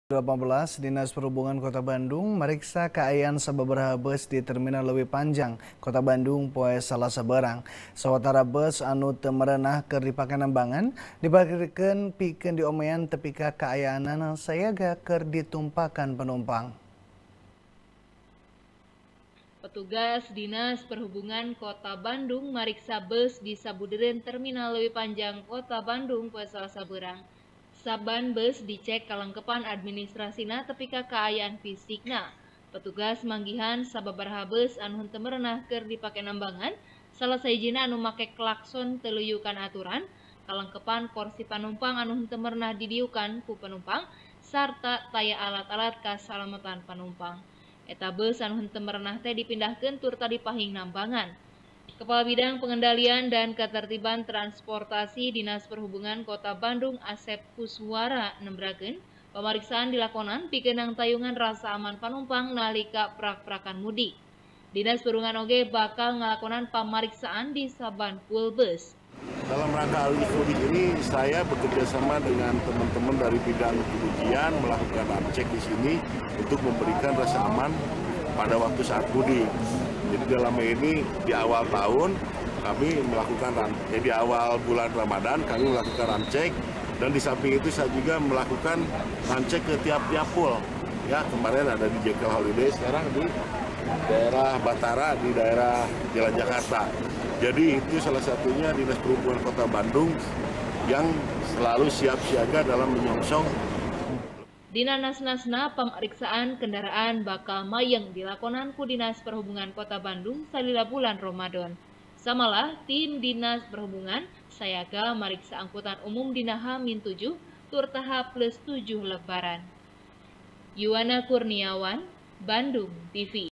DISHUB Mimiti Ngecek Kaayaan Beus | TP | BANDUNG TV
TP | BANDUNG TV | DERI FIRMAN | **   mapag arus mudik lebaran dua rebu dalapan belas, dinas perhubungan kota bandung mariksa kaayaan sababaraha beus, di terminal leuwipanjang, kota bandung, poe salasa beurang. sawatara beus anu teu merenah keur dipake nambangan, diparkirkeun pikeun diomean tepi ka kaayanana sayaga keur ditumpakan penumpang.